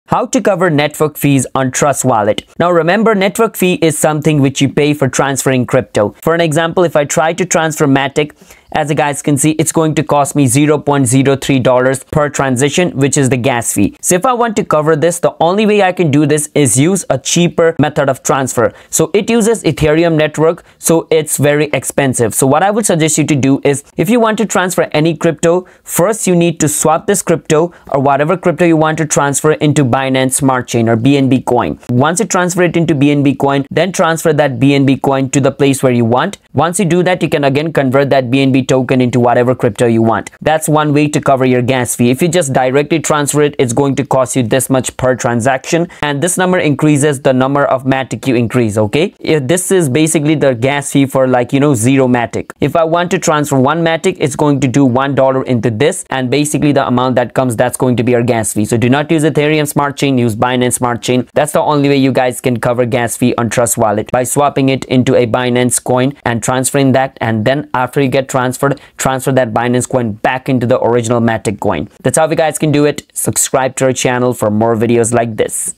How To Cover The Network Fees On Your Trust Wallet
This video walks you through the step by step process of how to cover the network fees on your Trust Wallet. Watch the video till the end and learn the steps in detail.